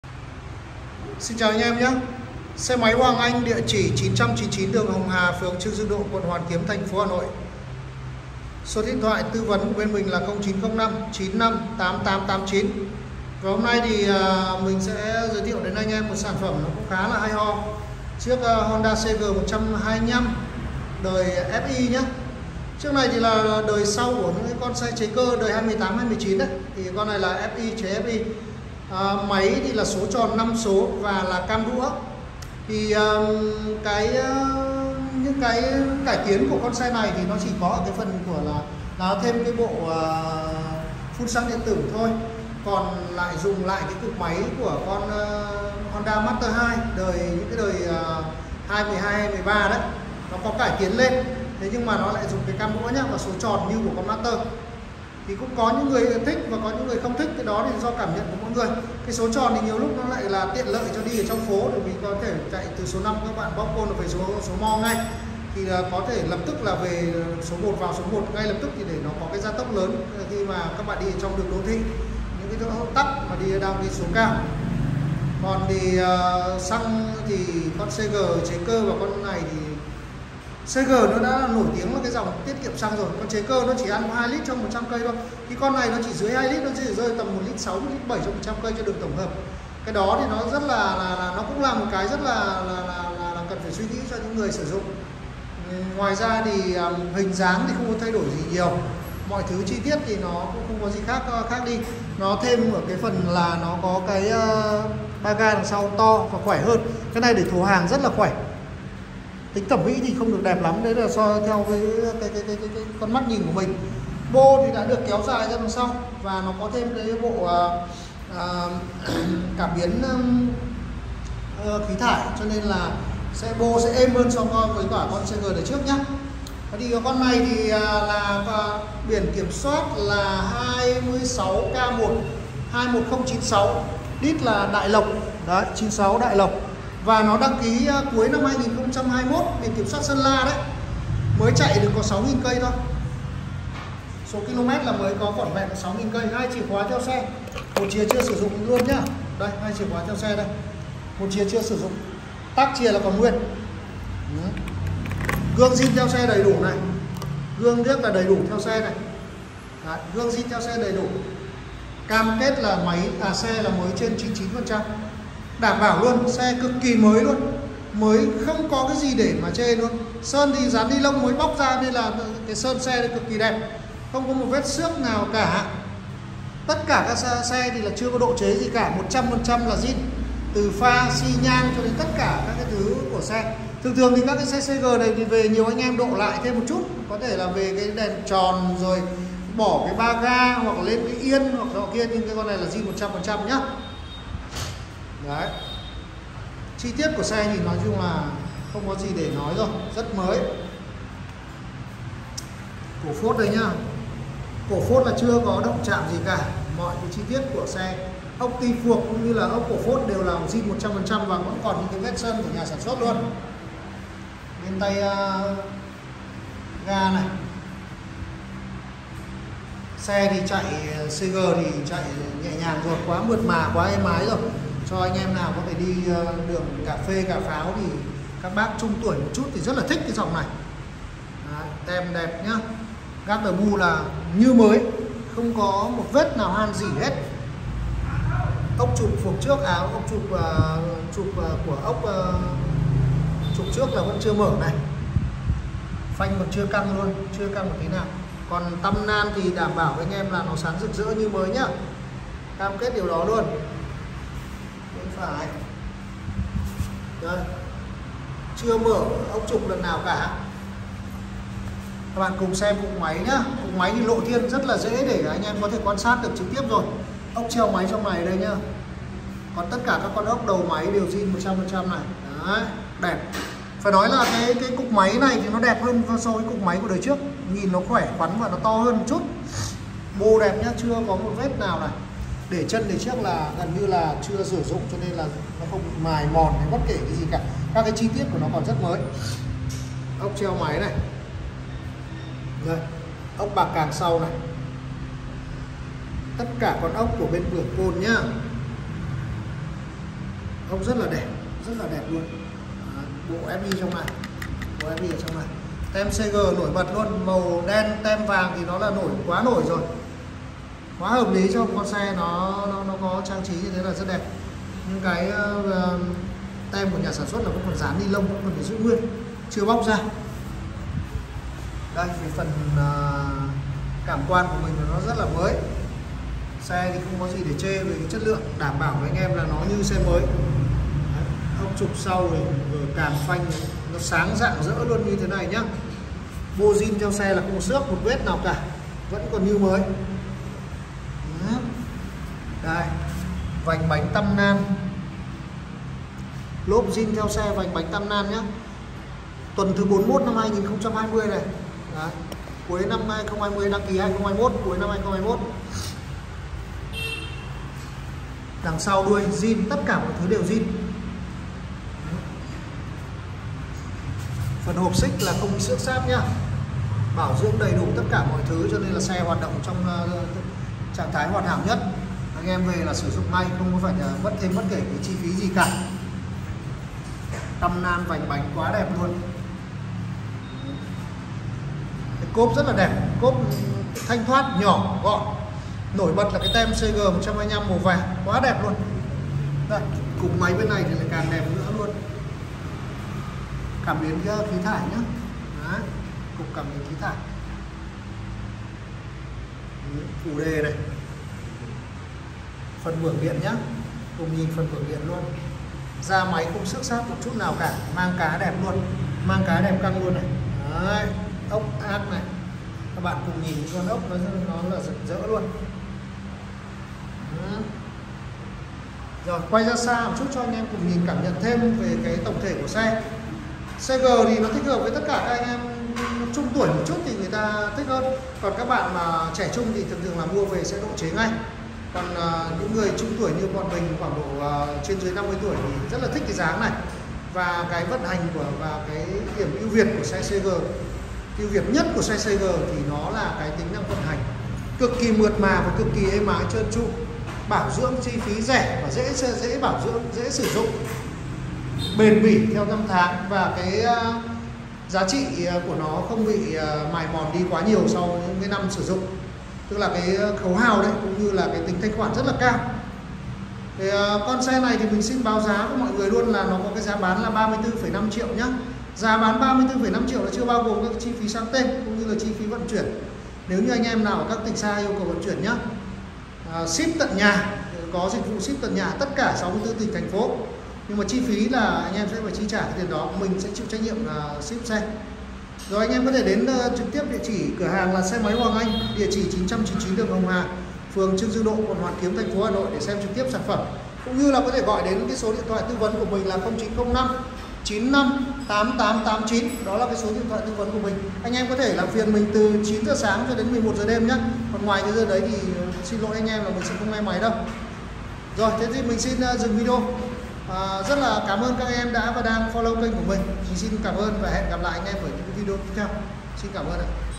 Honda Cg125cc phun xăng điện tử siêu lướt 6000km.DT 0905958889.Xe Máy Hoàng Anh 999 Hồng Hà,HK,HN
☀️ Honda CG125cc đời mới phun xăng điện tử. Dk 2021 mới như vừa khui thùng. Hai chìa khoá zin đầy đủ, sơn vừa bóc nilon như mới. giá như trong mơ có hơn 3 chục triệu.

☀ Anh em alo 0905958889

999HồngHàHoànKiếmHN — tại Xe Máy Hoàng Anh 999 Đường Hồng Hà, Hoàn Kiếm, HN.